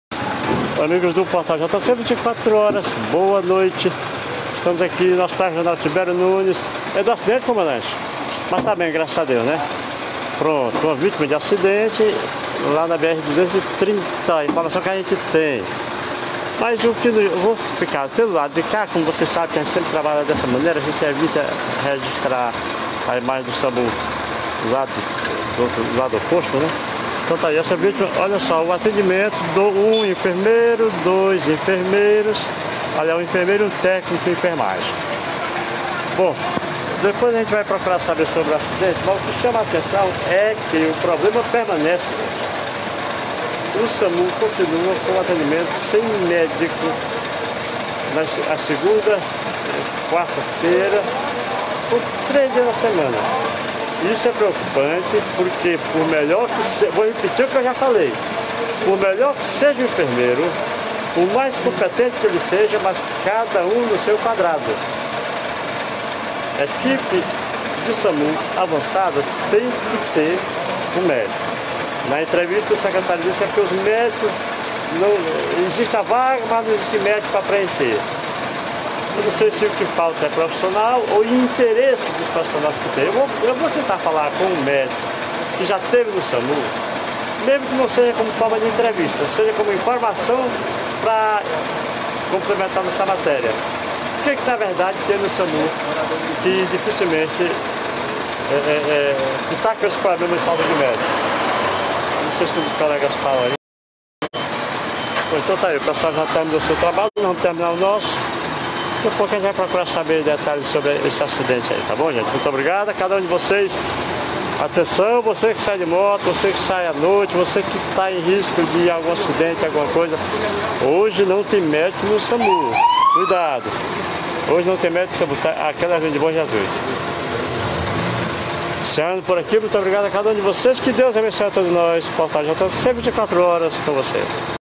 Samu é acionado para ocorrência de atropelamento, mas na verdade era embriaguez
Uma equipe avançada do Samu foi acionada para atender a uma ocorrência que, segundo informações dos populares era um caso de atropelamento, ocorrido na BR 230, nas imediações de um posto de combustíveis. Ao chegar no local, os profissionais de saúde constataram que na verdade, o homem caído estava embriagado e teria caído sozinho.
Com escoriações leves, Edson Barbosa Carreiro foi levado para o Hospital Regional Tibério Nunes e a reportagem do jc24horas registrou o momento de sua chegada, observando que a ambulância avançada não tinha médico em sua composição.